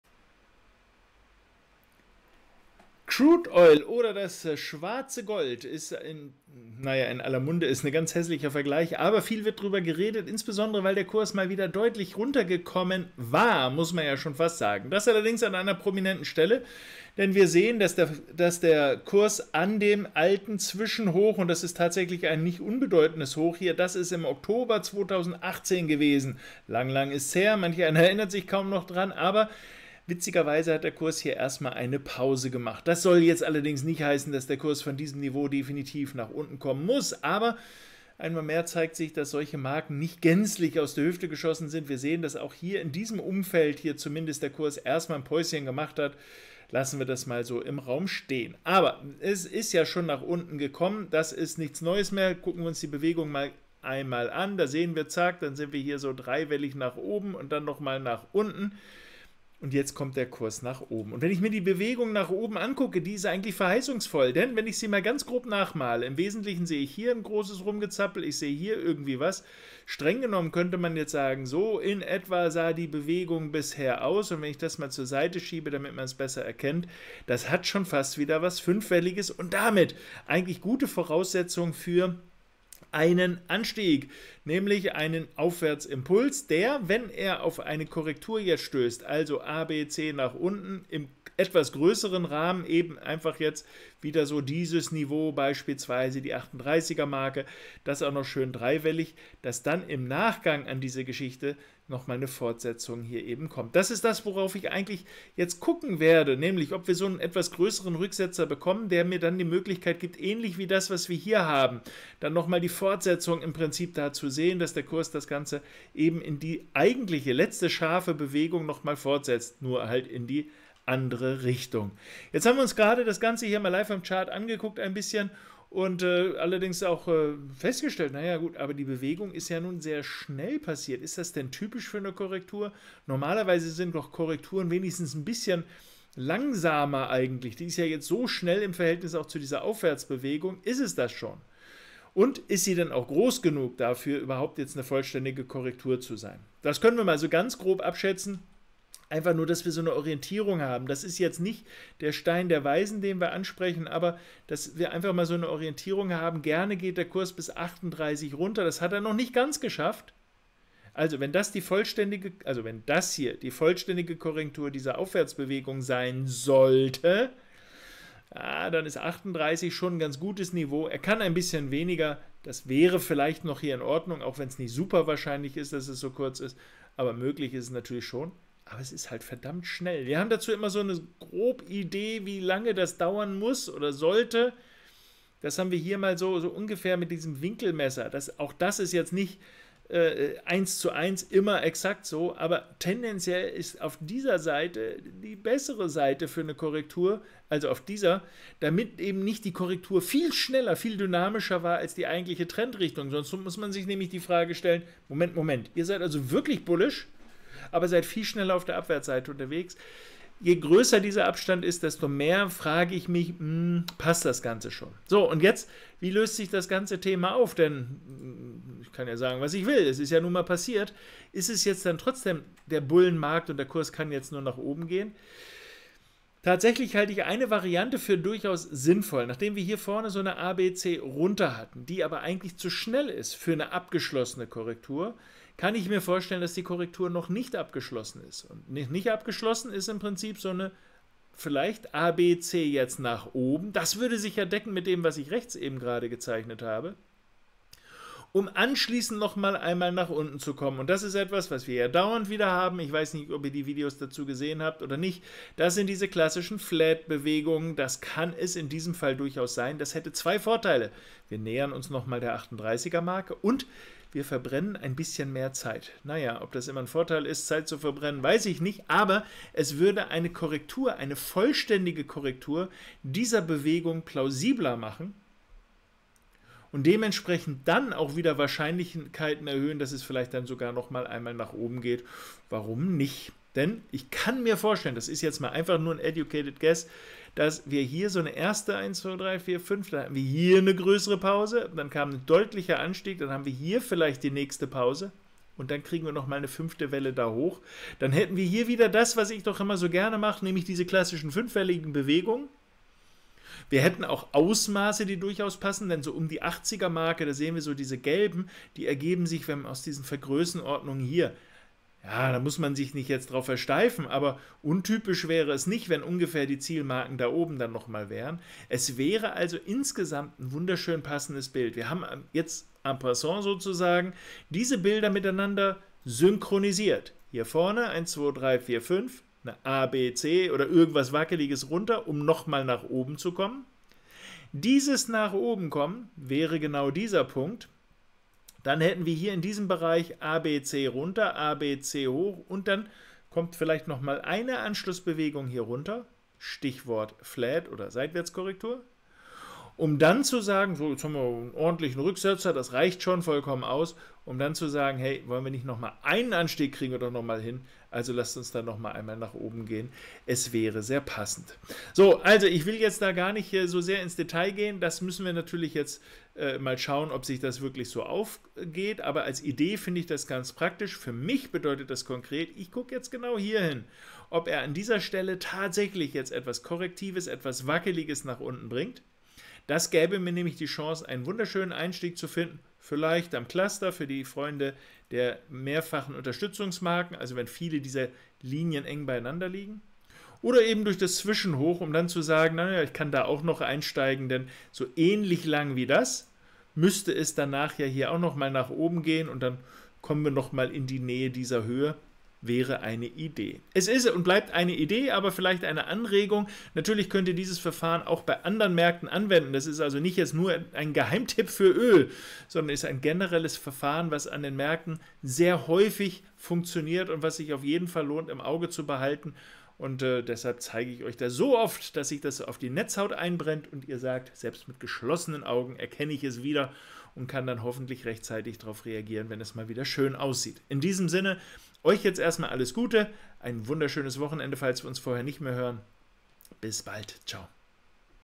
Rüdiger Born über hoffnungsfrohen Anstieg im Ölpreis
Der aktuelle Anstieg im Ölpreis sieht aus charttechnischer Sicht recht hoffnungsfroh aus im Sinne weiter steigender Kurse. Schauen wir im Video auf die Charts.